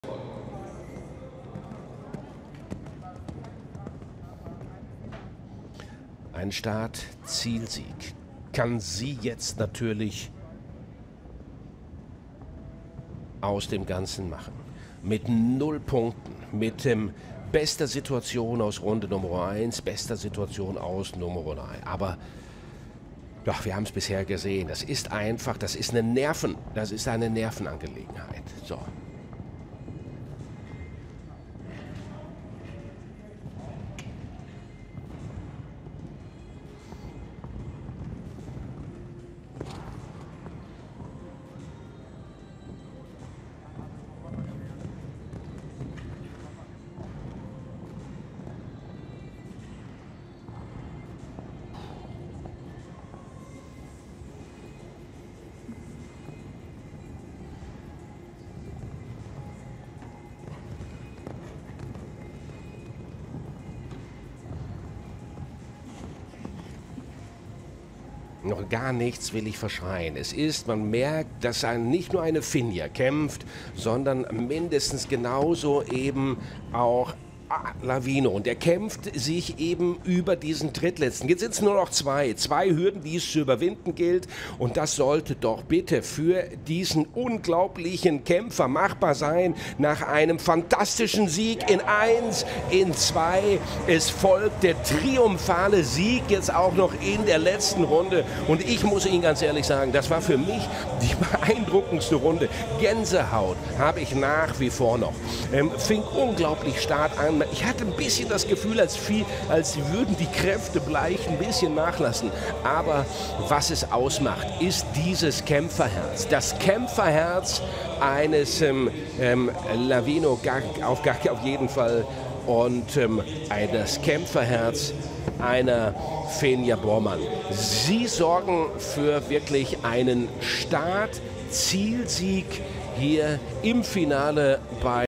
Finja Bormann ist Deutsche Meisterin 2020 🥇 | mit A crazy son of Lavina holt sie sich den Titel 🔝
Finja Bormann heißt die neue Deutsche Meisterin der Springreiter 💪🏼 Sie konnte sich mit A crazy son of Lavina den Titel holen 🥇